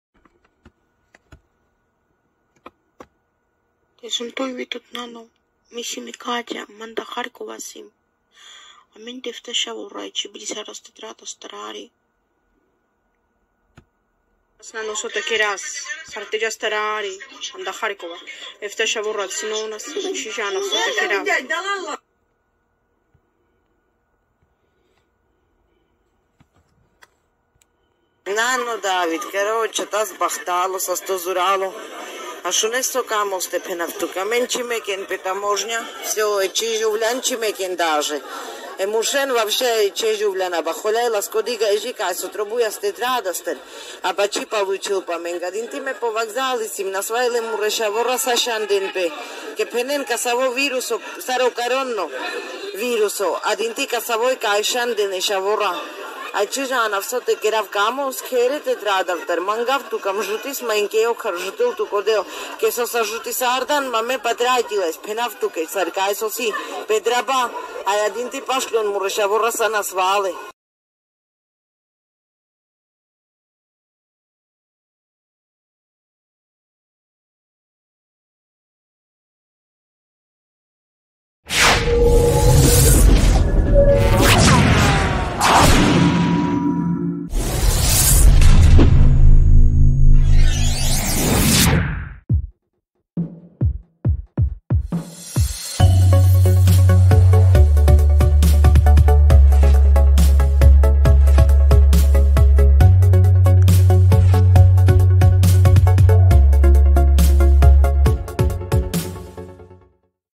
RNN Exclusive 2: 3 Romnja are begging for Help.
Audios of three Roma Women begging for help from Charkiv, Ukraine.

News and Information on the Roma Issues and Problems in the Ukraine Russia conflict. Powered by RomaNation and the RomNews Network.